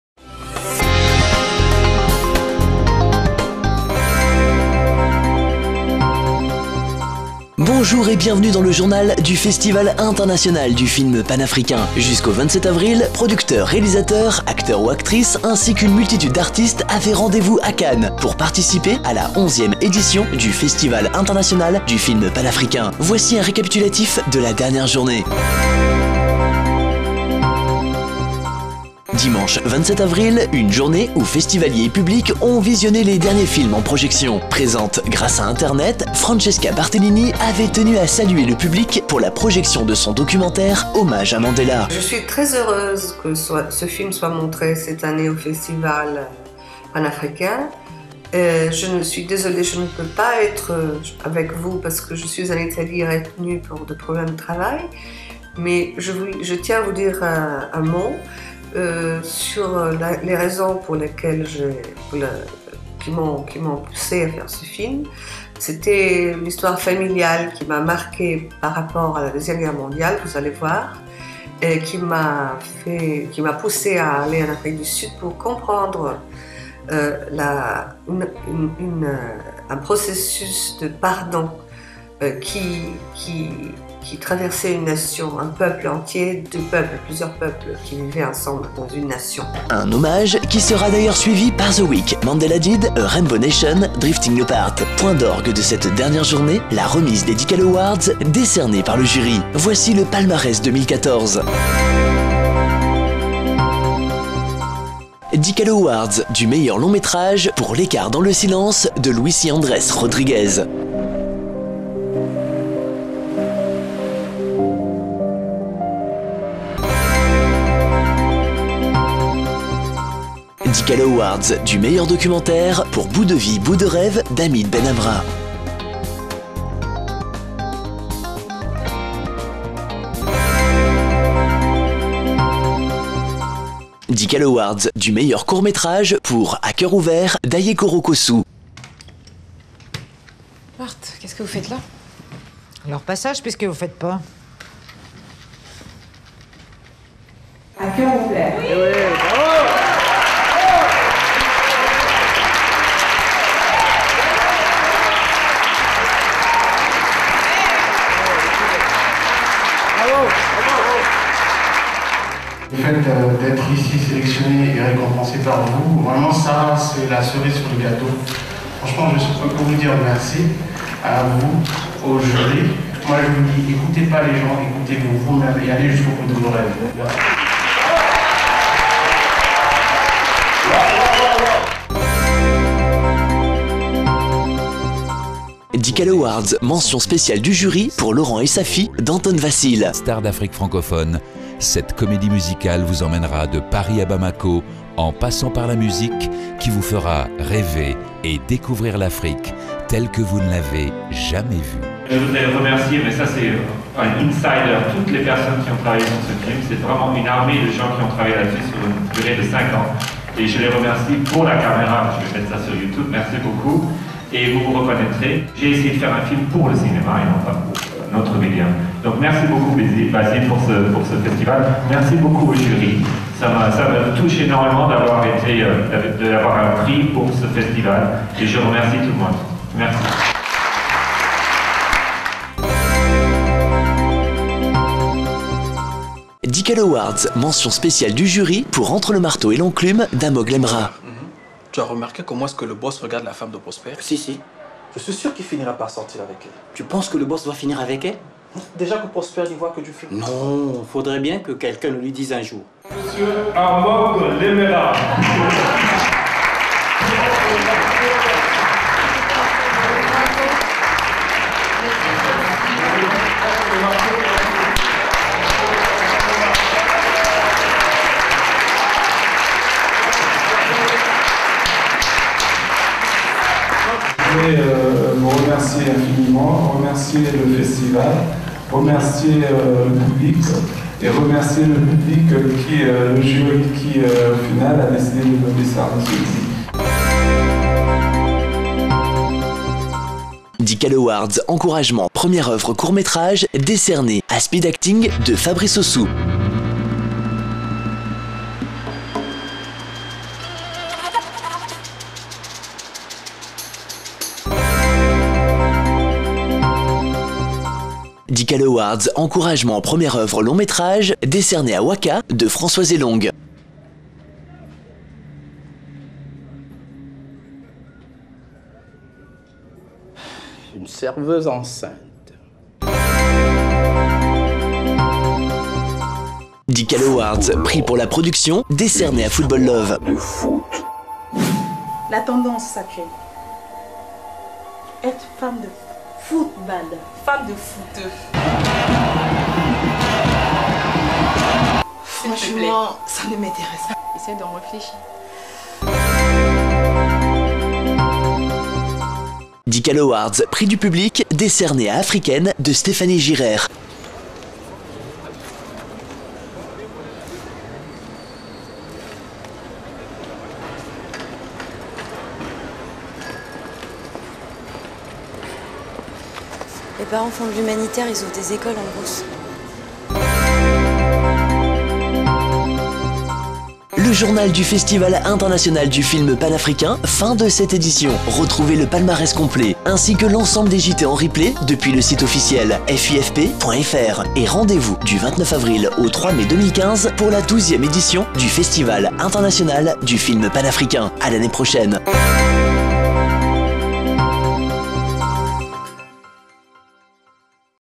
« JT FIFP 2014 » : récapitulatif du 27.04.2014
« JT FIFP 2014 » : récapitulatif de la journée du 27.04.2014. Du 23 au 27 Avril 2014 se tient la 11ème édition du Festival International du Film Panafricain à Cannes.
Chaque jour, retrouvez notre JT 100% « FIFP 2014 », à visionner dès à présent en replay, avec notre édition consacrée à la cinquième et dernière journée.
Au programme :
Flash complet sur les projections du jour ainsi qu'un point sur la cérémonie de clôture & le palmarès 2014.
Retrouvez toute l'actu du « FIFP 2014 » en direct de Cannes sur le site officiel
JT FIFP 2014 © DB2M Productions -- 2014